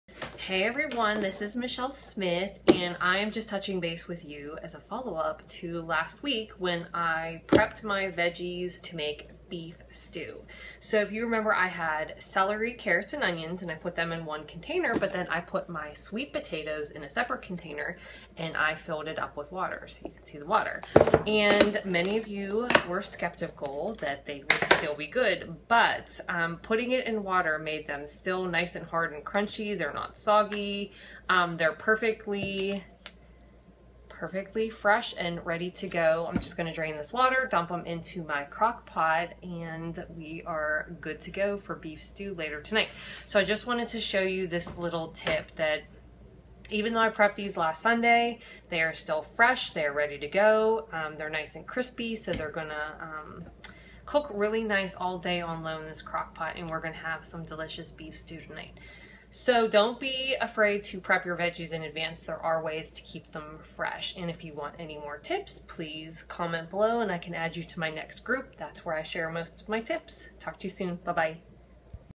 Meal Prepping Advice: Beef and Veggie stew in advance
I always plan a crockpot meal on Sunday's and Sunday's are always my shopping and prepping day. So, how do I make it easy to get my regular planning and prepping done without having to also prepare my Sunday meal? I prep the week before. Did you just gasp? Seriously, I do!

I prepped veggies and beef for stew last Sunday. I froze the trimmed and cubed beef and stored the veggies in resealable containers. I separated my sweet potato from the other veggies because I stored it in water to keep it super fresh and crispy. Don't believe me?

If you like this tip or want this recipe check me out The best way to get regular tips and advice is to join my next online group where I share tips and recipes everyday!